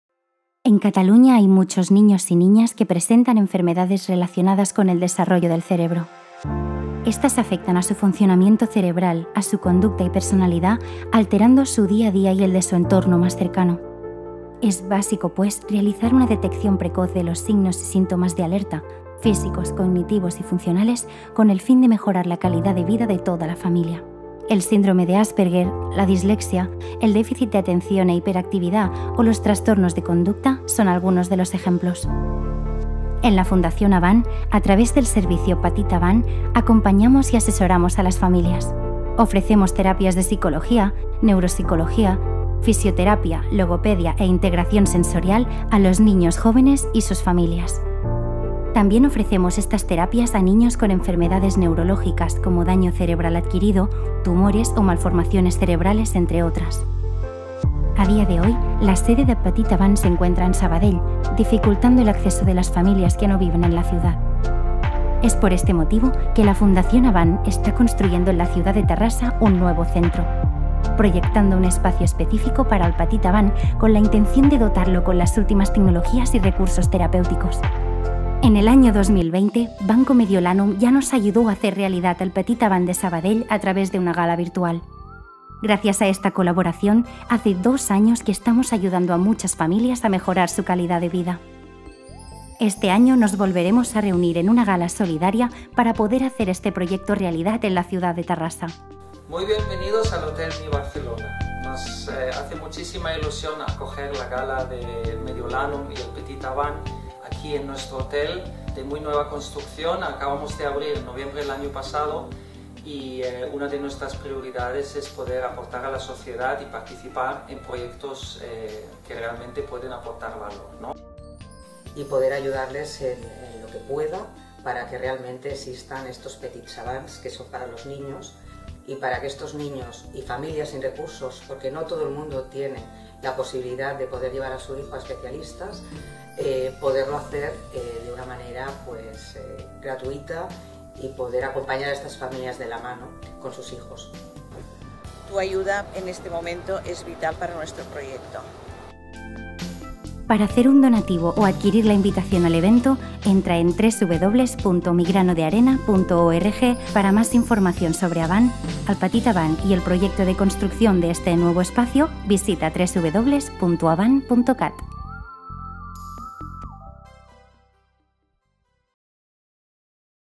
Gala Solidaria Mediolanum x Fundació AVAN
El proximo 3 de noviembre tienes una cita en el Hotel Me by Melià de Barcelona con Mediolanum Aproxima.
Juntos haremos posible el proyecto Petit AVAN en Terrassa, un centro de atención integral para niños y jóvenes con enfermedades neurológicas, trastornos del neurodesarrollo o necesidades educativas específicas.